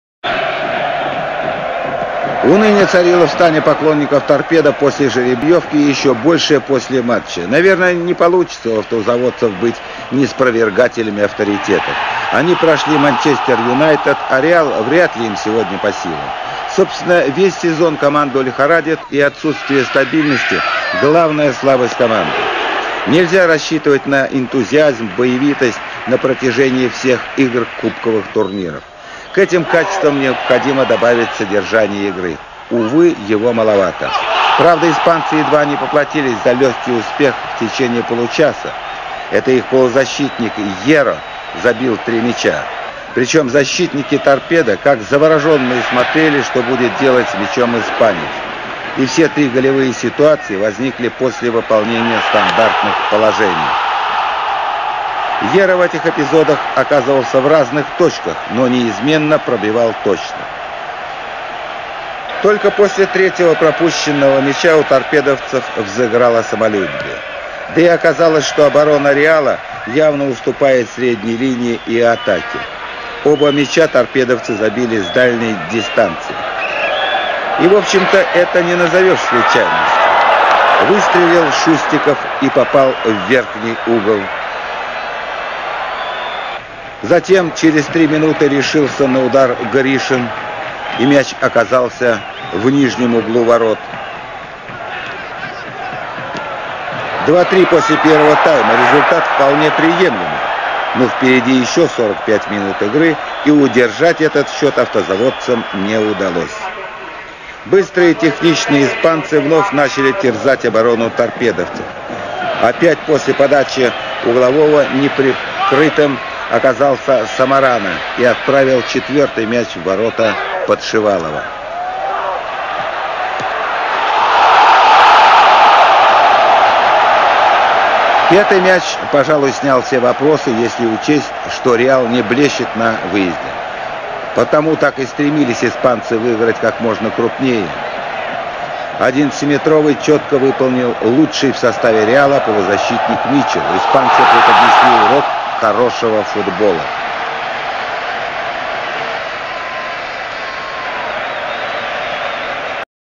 Реал Мадрид 5-2 Торпедо. Кубок УЕФА 1992/1993
Реал (Мадрид, Испания) 5-2 Торпедо (Москва)
Кубок УЕФА 1992/1993. 1/16 финала. Первый матч.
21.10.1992
Мадрид, стадион "Сантьяго Бернабеу"